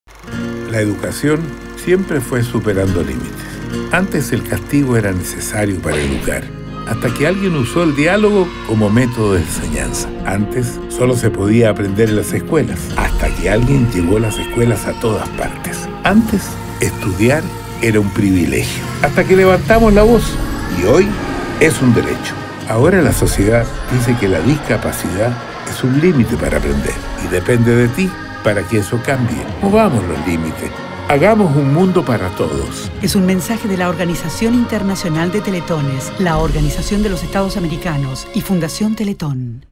La educación siempre fue superando límites.
La sociedad siempre puso límites a las personas, los va imponiendo de alguna manera sin que nos demos cuenta. Antes se limitaban los derechos según las razas. El acceso a la educación según la clase social. Se limitaba el acceso de la mujer al trabajo y al voto.

La gran historia de la humanidad ha sido la lucha continua por sus derechos. La lucha por la dignidad, la libertad, la igualdad de razas, los géneros, las edades; la lucha por liberarse de la tiranía, la crueldad, la discriminación, la ignorancia y la pobreza.

Ahora la sociedad dice que la discapacidad es un límite. Y nosotros estamos seguros de que la discapacidad no es de la persona sino de la sociedad que la acentúa.

Unite a la discusión usando movamosloslimites y ayudanos a que esto cambie.

Movamos los límites. Hagamos un mundo para todos.

Periscope: teletonparaguay